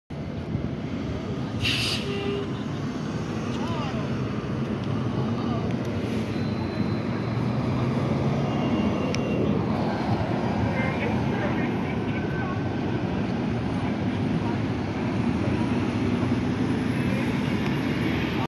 Columbia University 2018 New Flyer XE40 #2995
Spotted in New York
Motor: Siemens ELFA2
Energy Storage: XALT Energy

Operated by Academy Bus Lines

Flickr - SoCal Transit Studios
CPTDB - Newflyerxn40